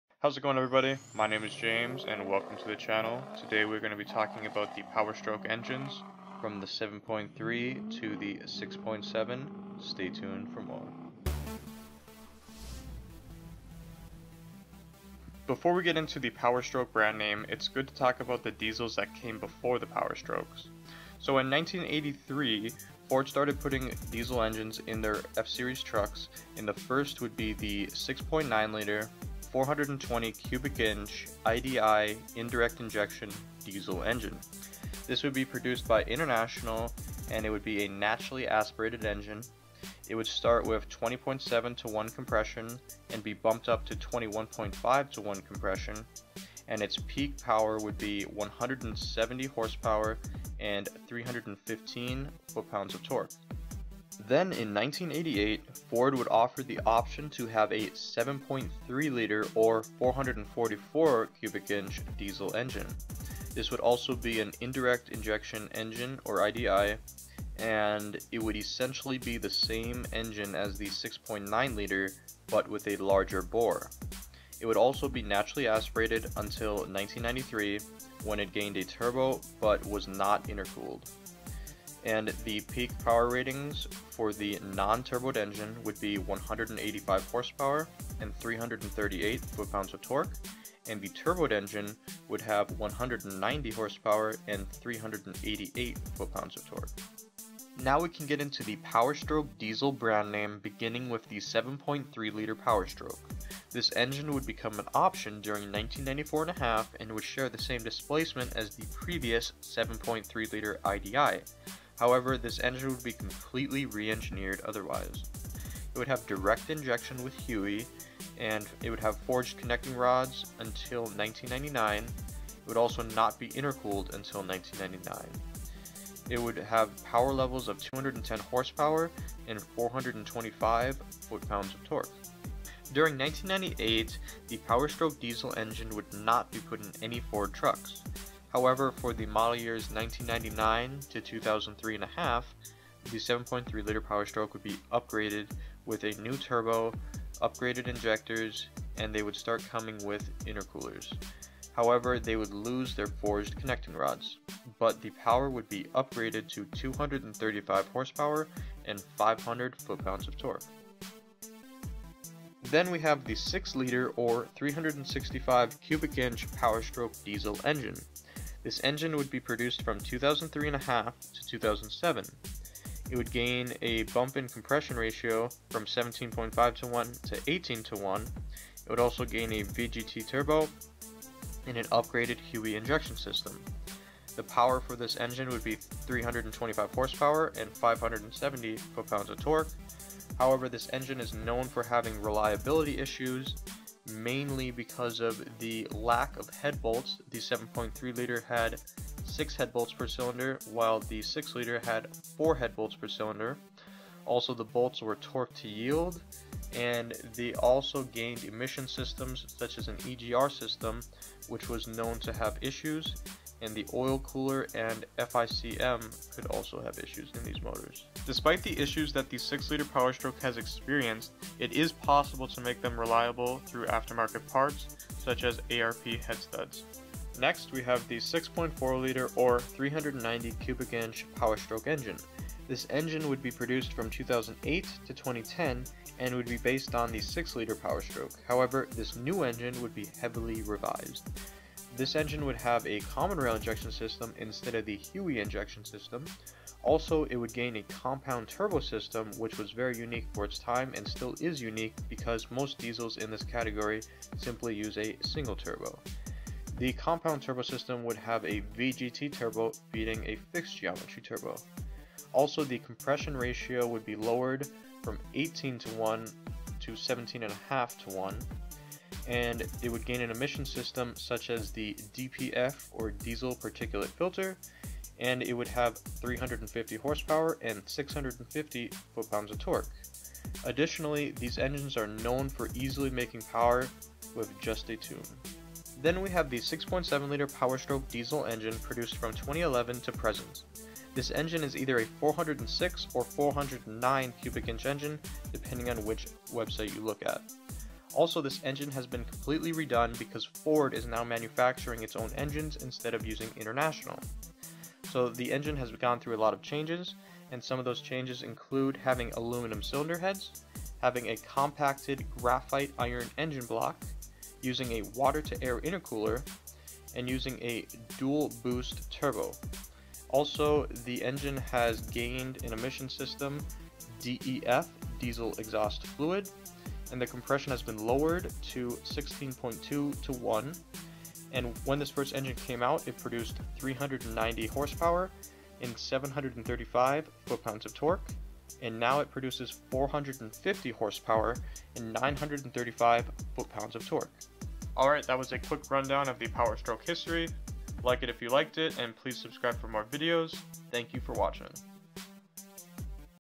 History of the Powerstroke Diesel: From 7.3L to 6.7L
This is a quick history video about the Powerstroke diesel engines including the 7.3 liter, 6.0 liter, 6.4 liter, and 6.7 liter.